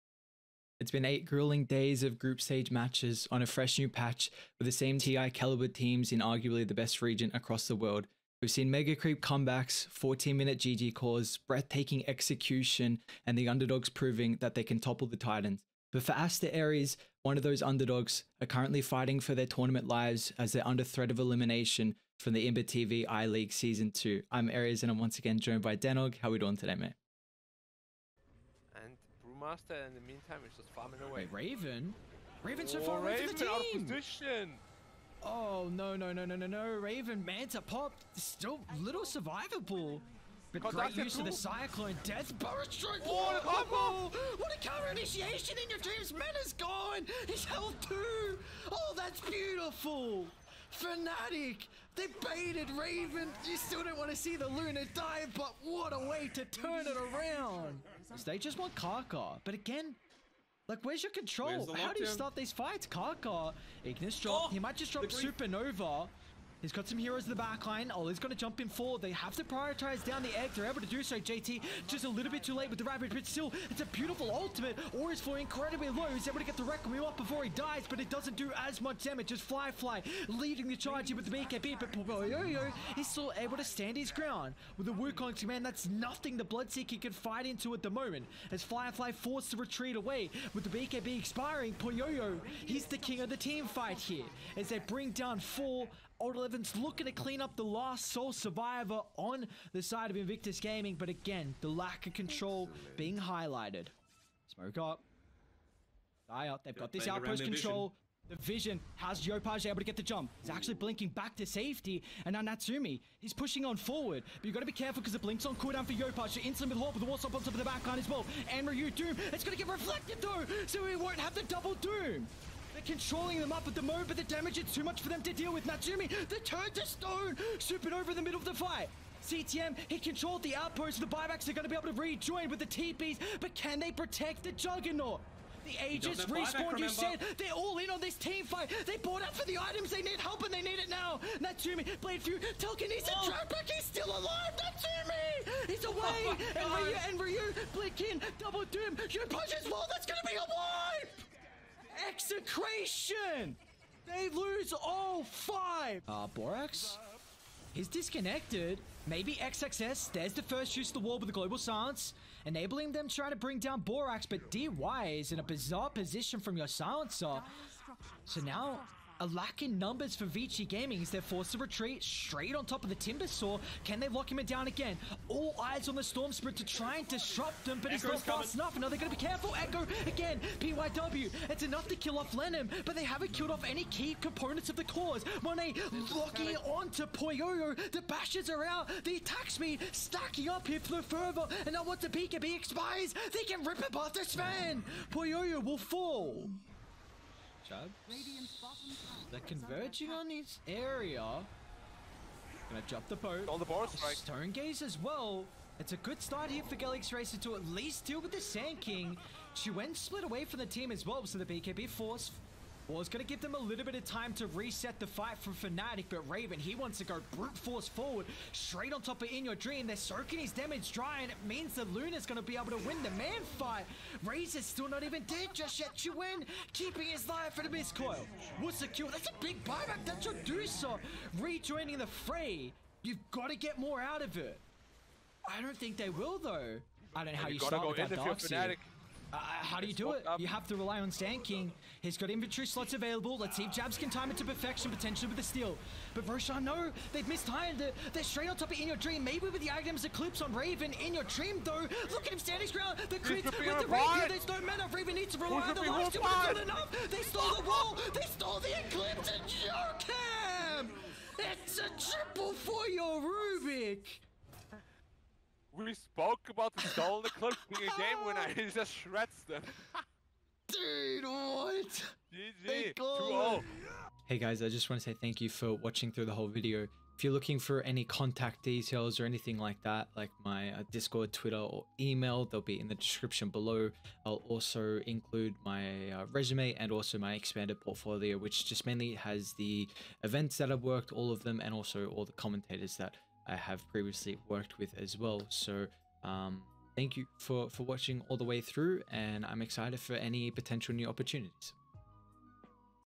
Casting Reel 2.0
Hey guys!
Another little casting highlight reel that I wanted to put together to showcase my talent. 2021 has been crazy and I'm so excited to continue growing into the future.